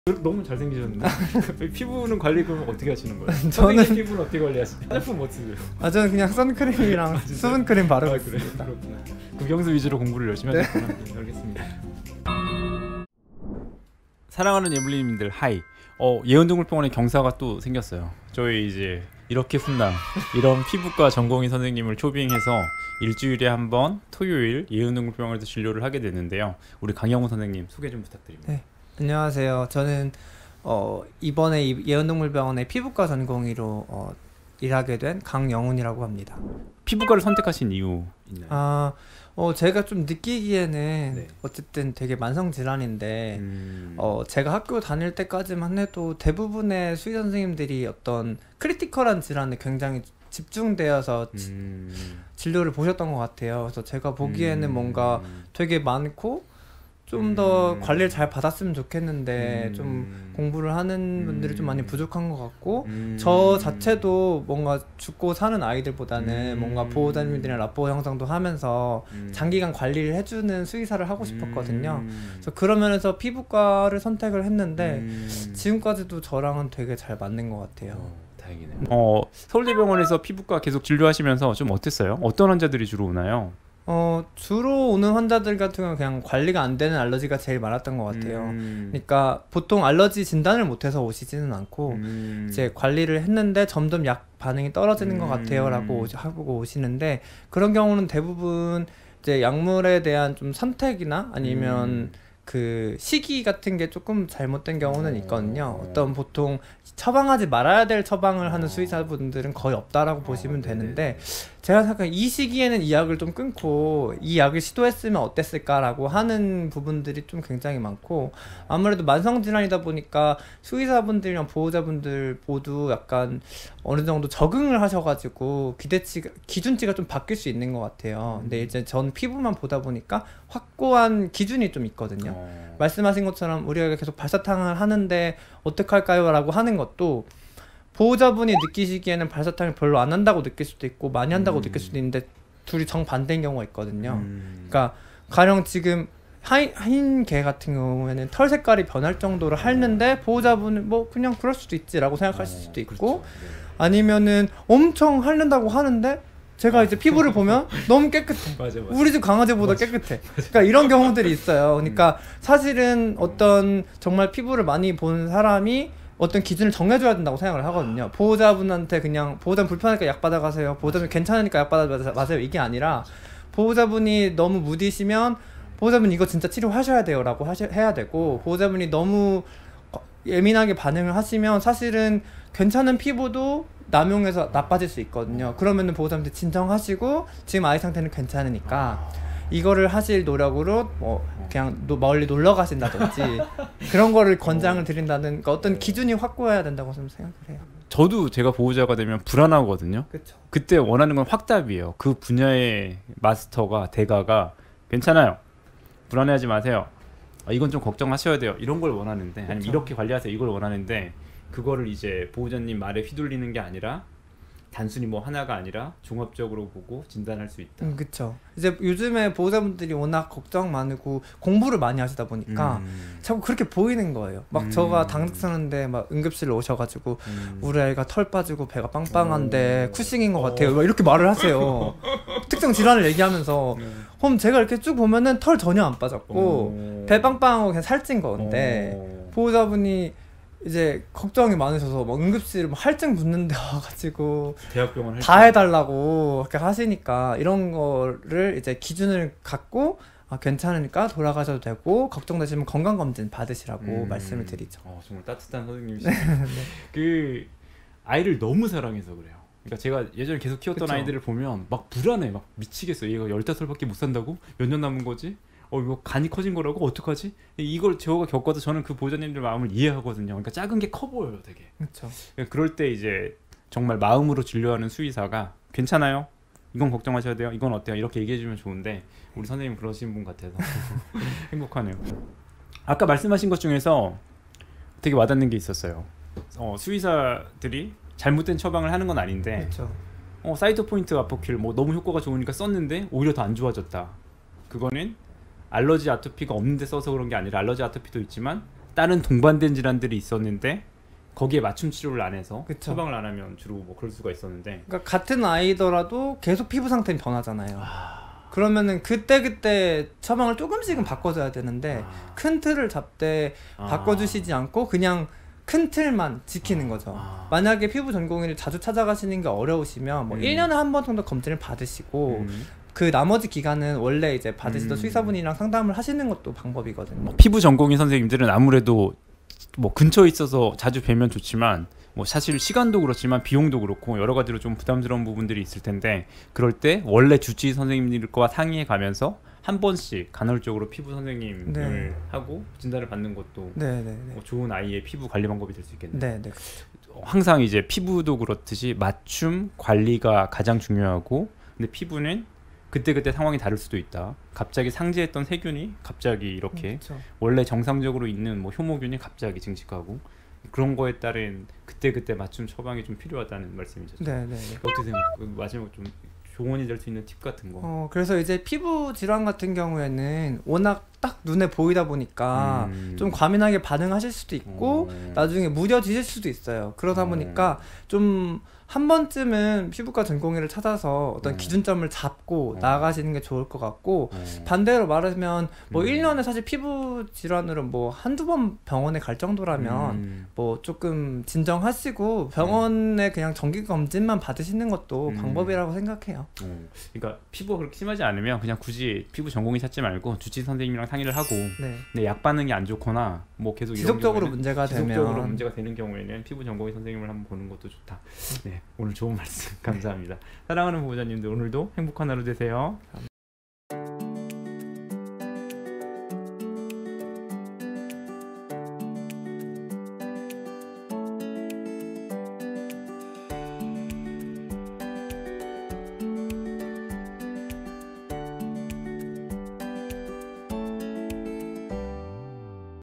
반려동물 피부과 강영훈 수의사 인터뷰
안녕하세요. 예블리분들♥
오늘 영상은 많이들 궁금해하셨던 피부과  강영훈 수의사 선생님 인터뷰에요!♥

아이들 피부만  진료하는 교수님이 계시다는 사실 알고 계셨을까요?

앞서 커뮤니티로 안내드린바와 같이 세계적으로 인정받는 우리나라
피부 진료 일인자 서울대 황철용 교수님께  수련받은 강영훈 수의사 선생님을 초빙 했습니다.

황교수님께 박사과정 수료하신 몇안되는
분이에요

다음주 토요일부터 매주 토요일 12시-9시 진료를 하게 됩니다.

난치성 알러지 피부병이나 사이토포인트/아포퀄등의 처방에서 대학병원과 동일한 프로토콜로 치료예정이에요.

아토피에서 대학병원과 동일한  검사와 약물로 면역치료도 가능 합니다

피부 진료 원하시는 보호자님들께서는 예약 부탁 드립니다

우리 예블리분들 오늘 하루도 따뜻하고 행복한 주말되세요♥